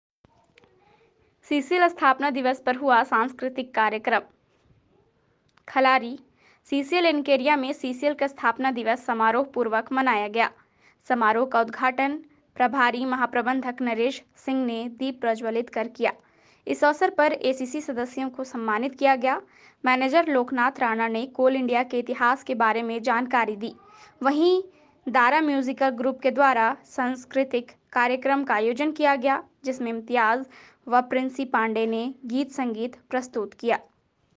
सीसीएल स्थापना दिवस पर हुआ सांस्कृतिक कार्यक्रम
खलारी।सीसीएल एनके एरिया में  सीसीएल का स्थापना दिवस समारोहपूर्वक मनाया गया।समारोह का उदघाटन ने प्रभारी महाप्रबंधक नरेश सिंह ने दीप प्रज्वलित कर किया।इस अवसर पर एसीसी सदस्यों को सम्मानित किया गया।मैनेजर लोकनाथ राणा ने कोल इंडिया के इतिहास के बारे मे जानकारी दी।वही दारा म्यूजिकल ग्रुप के द्वारा सांस्कृतिक कार्यक्रम का आयोजन किया गया।जिसमें इम्तियाज एव प्रिंसी पांडेय ने गीत संगीत प्रस्तुत किया।